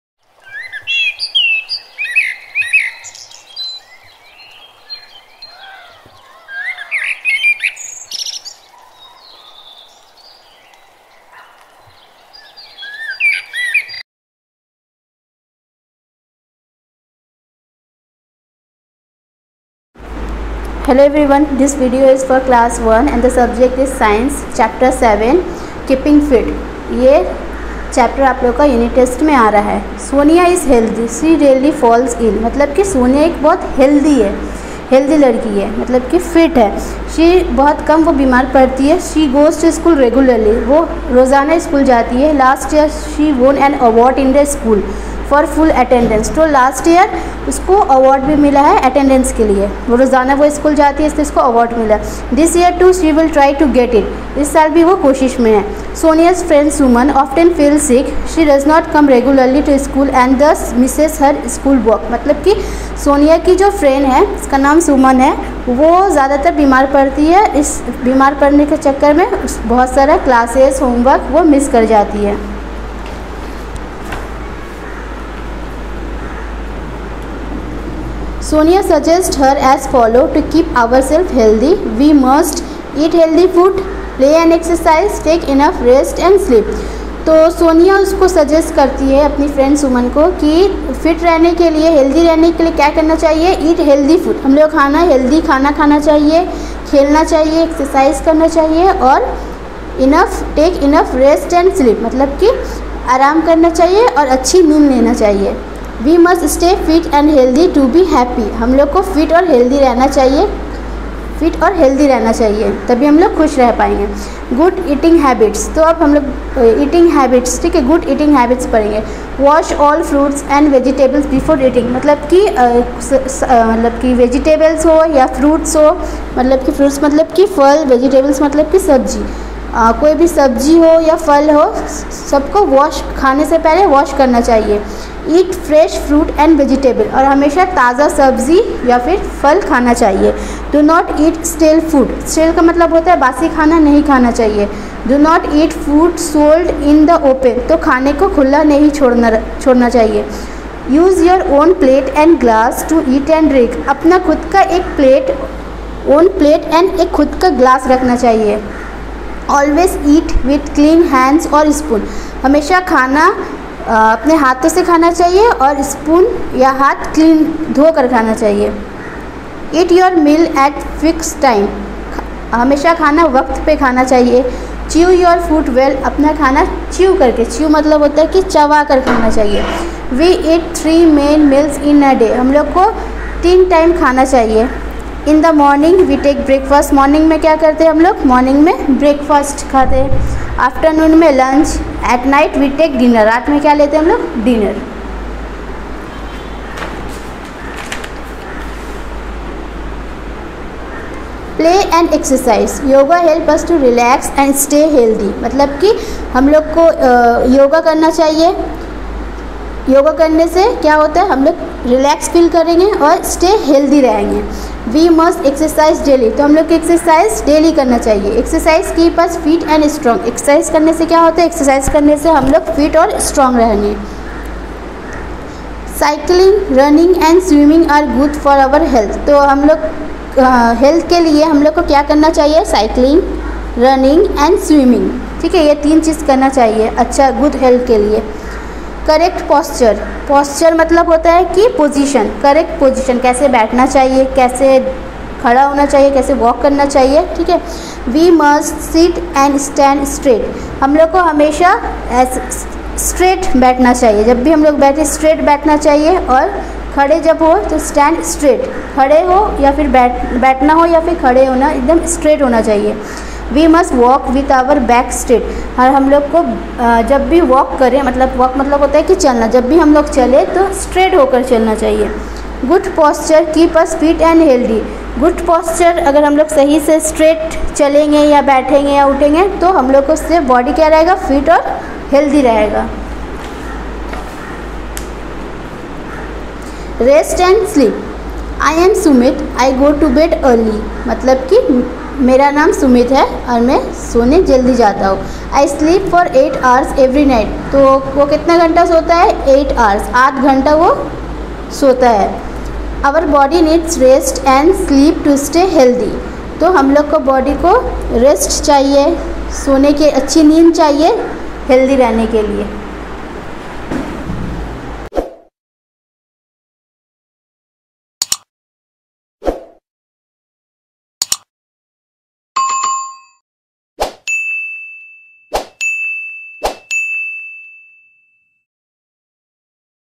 Keeping Fit, Health, Science, For class One (I)
This is an educational video for Class One (I) students.
Here the following topics are covered:
Topic:   Keeping Fit
Subject: Science